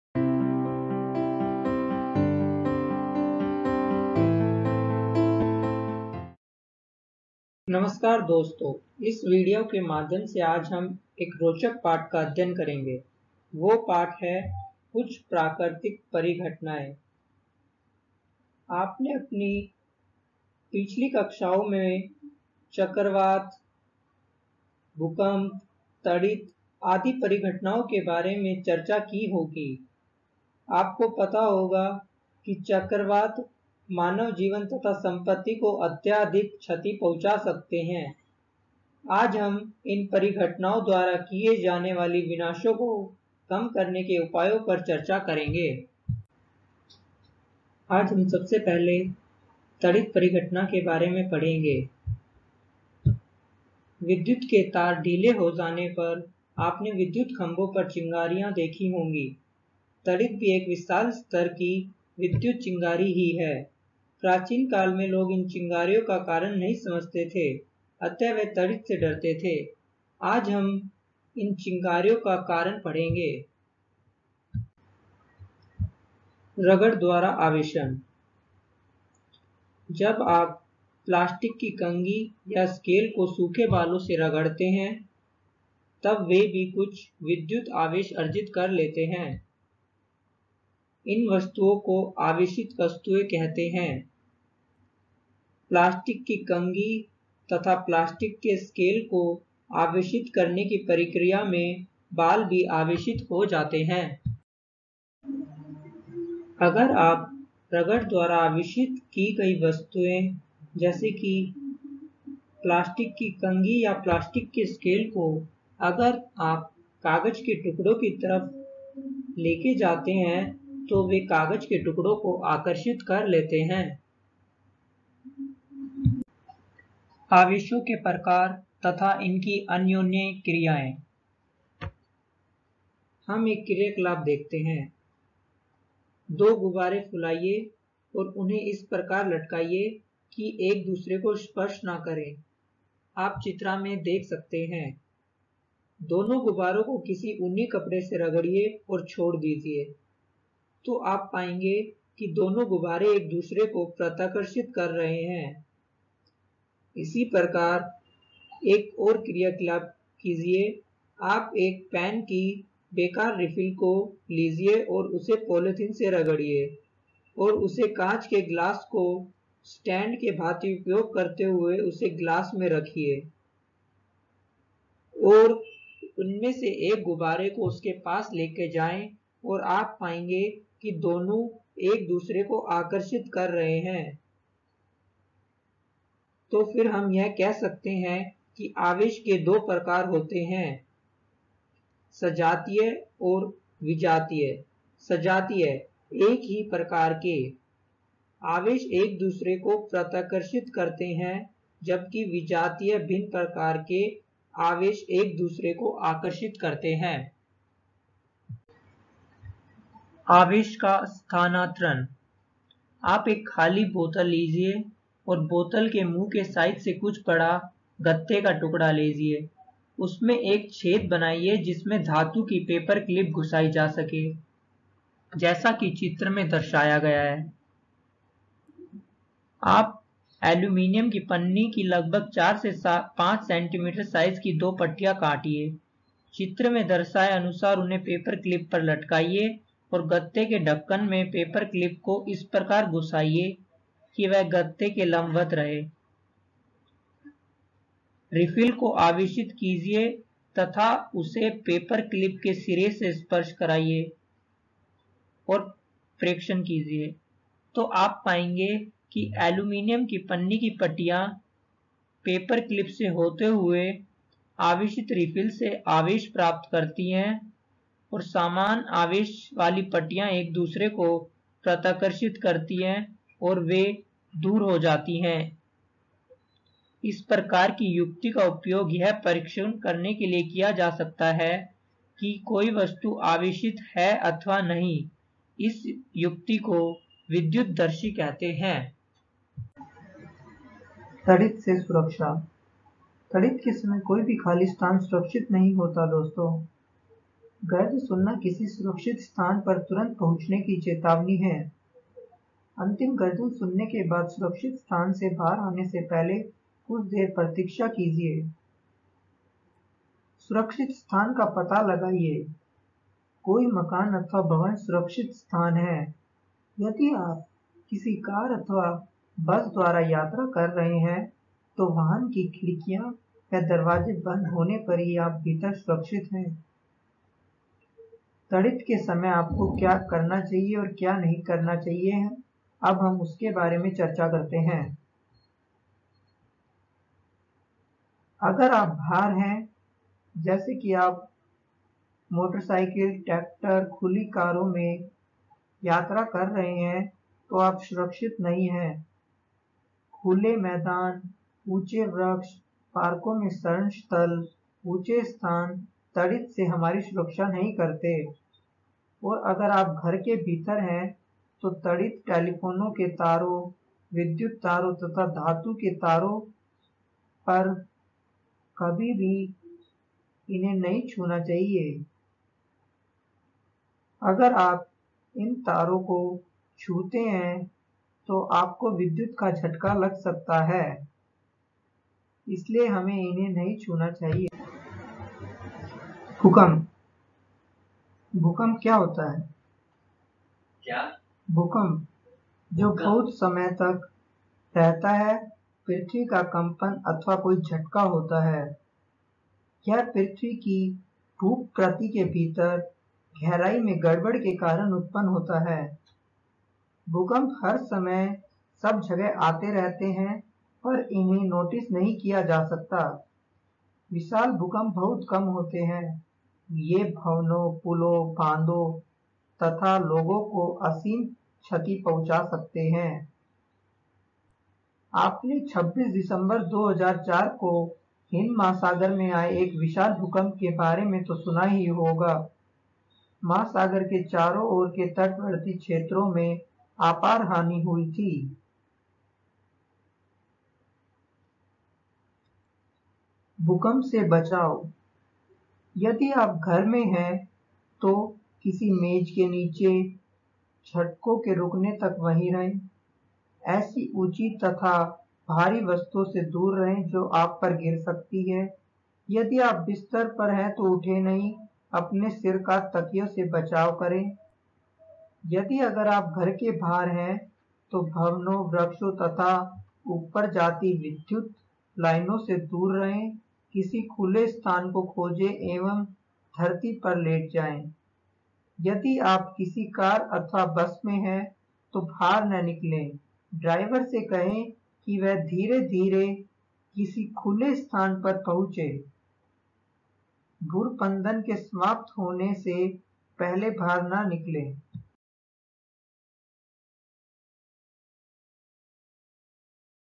कुछ प्राकृतिक परिघटनाएँ (Some Natural Calamities) - कक्षा 8 विज्ञान (Class 8 Science) - Hindi
This video begins with lightning and explains the principles of electrostatics that cause lightning.  It then explains the safety measures that can be followed in case a lightning strike is expected. The next calamity discussed is earthquake and the safety measures to be employed in case of an earthquake.

Taught by: Nilesh Aggarwal